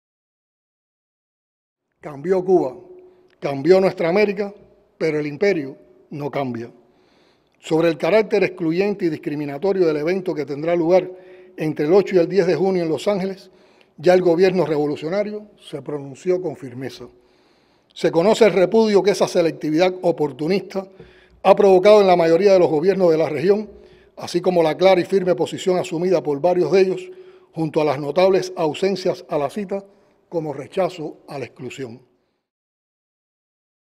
La Cumbre de las Exclusiones
Nuestros principios son sólidos, la voz de Cuba siempre será escuchada en cualquier foro internacional